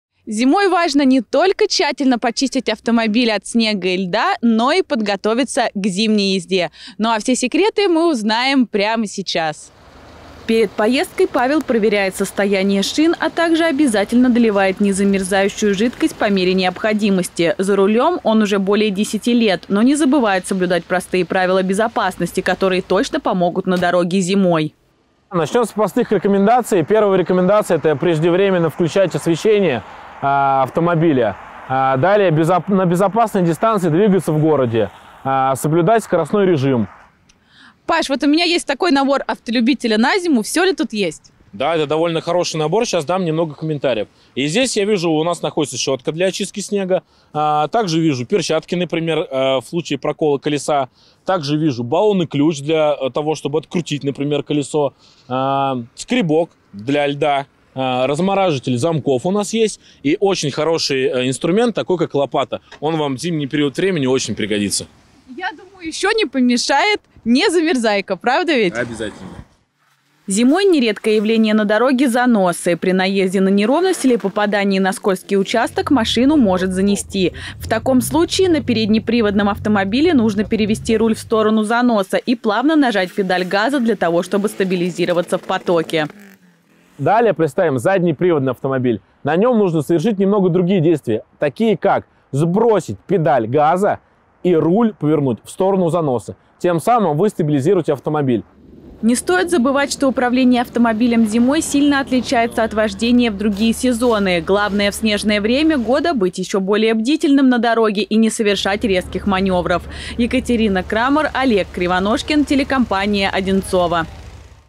Как обезопасить себя на дороге
Короткий световой день, снежные заносы, плохая видимость из-за непогоды. Чтобы снизить свой риск на дороге, необходимо выполнять несложные правила, которые обезопасят вождение автомобиля зимой.